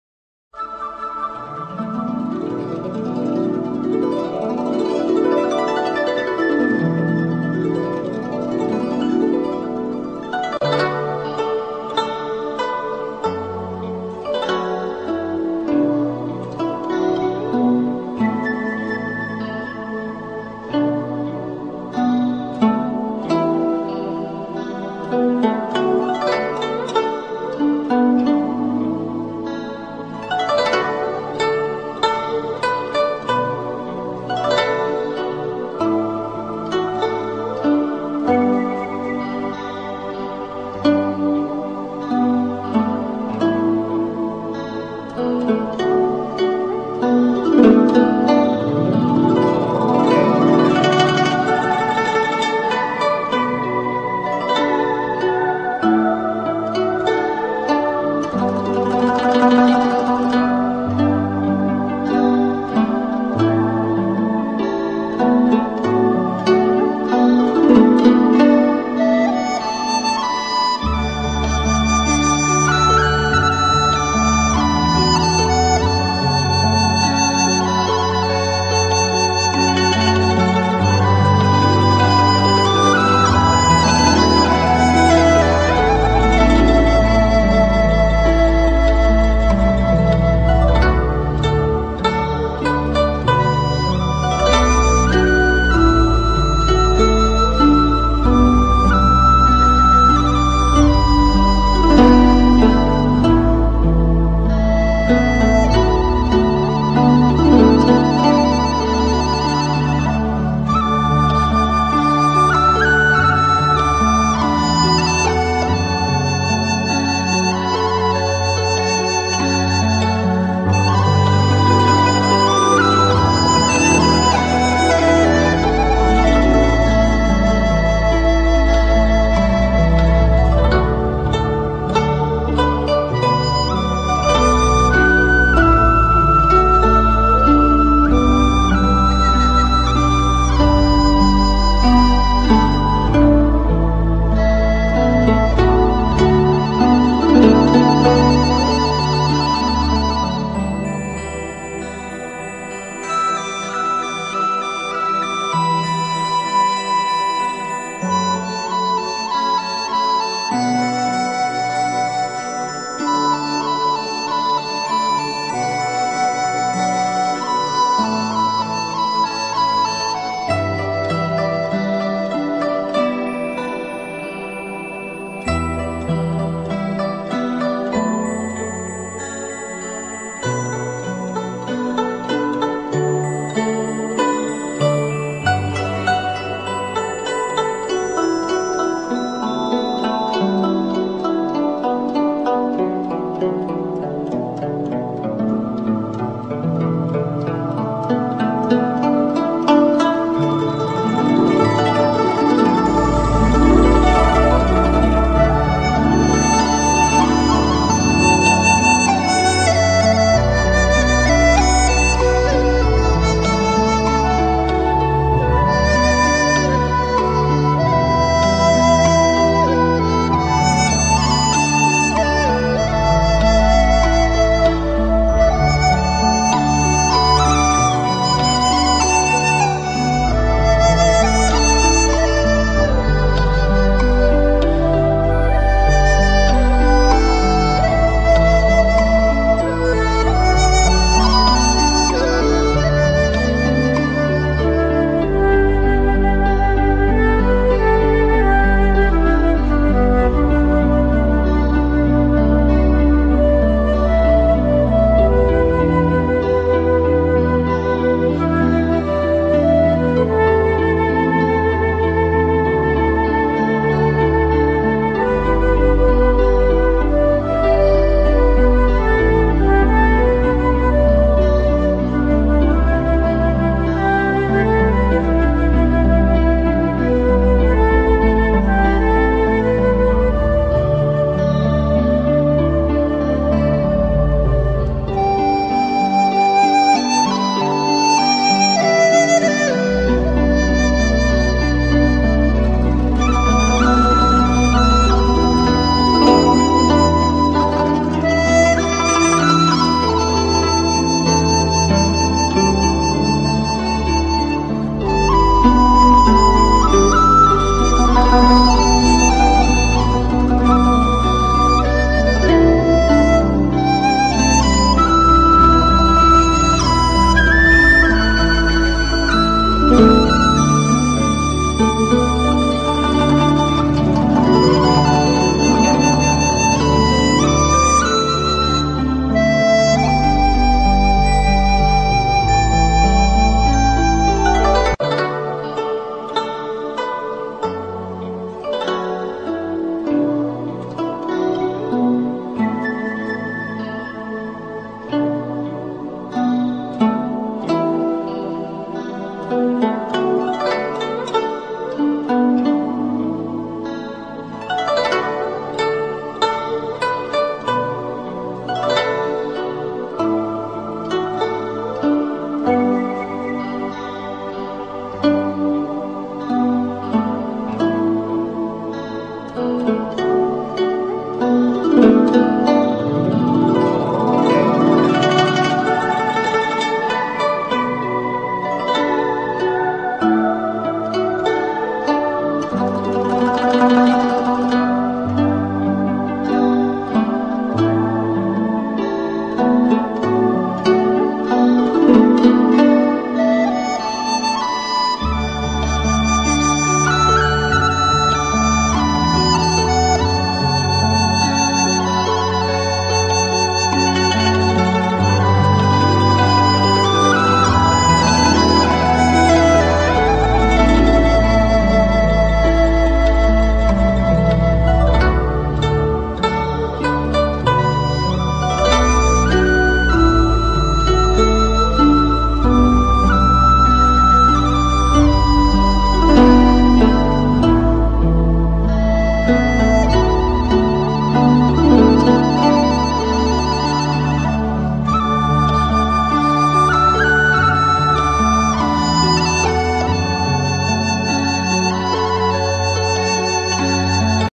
JiangsuVsLiaoning Highlights 龚翔宇|Game 2|ChinaVolleyball League 2019/2020
本场获得18分，稍感疲倦。世俱杯来临，请保护好自己。This game obtains 18 points, slightly feels tired. Club World Cup is coming, please protect yourself.